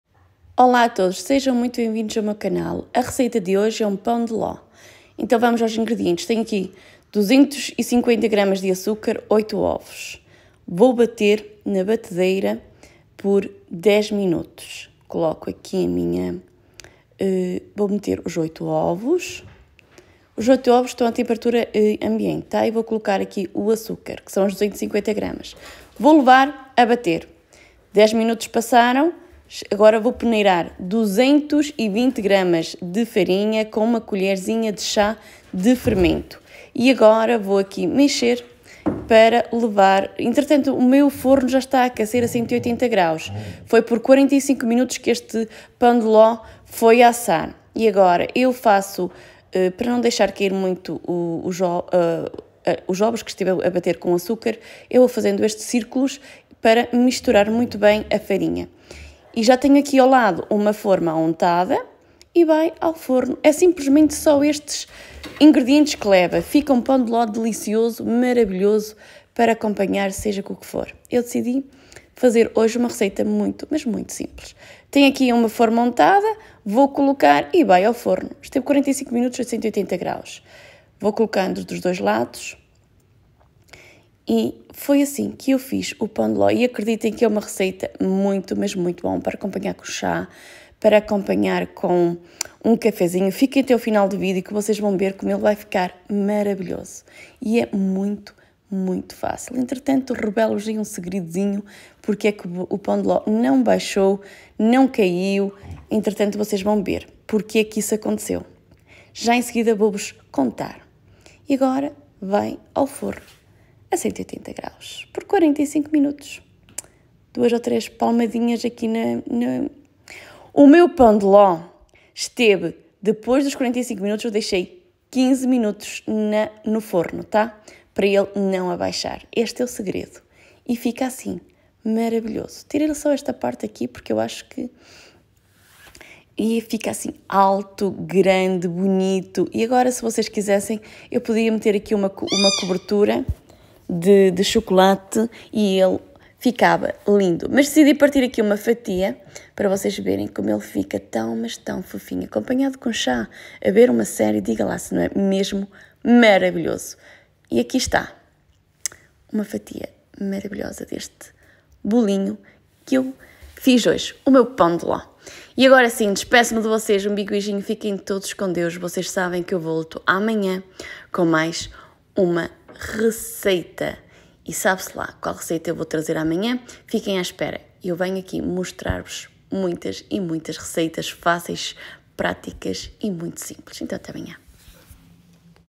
COMO FAZER PÃO DE LÓ COM APENAS 4 INGREDIENTES | receitas da susana correia
Para saber como eu fiz essa delicioso pão de ló, assista o vídeo até o final. Obrigada, big beijinho!

Olá a todos. No vídeo de hoje ensinarei a vocês a receita de como fazer delicioso pão de ló. Acompanhe o passo a passo que não tem erro. É muito prático e simples de fazer e irão chorar por mais!!!

Deixarei abaixo os ingredientes necessários. 👇🏼

🧾  Ingredientes:
- 250g de açúcar;
- 8 ovos;
- 220g de farinha;
- 1 colherzinha de chá de fermento.

*180 graus por 45 minutos.

📖  Passo a passo:
1. Deixar o forno a aquecer;
2. Meter os ovos e o açúcar na batedeira e bater por 10 minutos;
3. Peneirar 220g de farinha e 1 colherzinha de chá de fermento, misturar muito bem;
4. Untar a forma, colocar a massa e levar ao forno por 45 minutos a 180 graus;
5. Servir.

00:00 Introdução
00:06 Ingredientes necessários
00:15 Modo de preparo
01:15 Colocando a massa na forma
02:03 SEGREDO ESPECIAL
02:07 Tirando o pão forno
02:37 Está pronto. Veja como ficou!!!

👍🏼 Para mais receitas, inscreve-te no meu canal e ativa as notificações para estares sempre a par das novidades! 🤭

Lombo de Porco Assado com Cerveja: Como Deixar seus Convidados com Água na Boca | susana correia

susana correia receitas rapidas receita caseira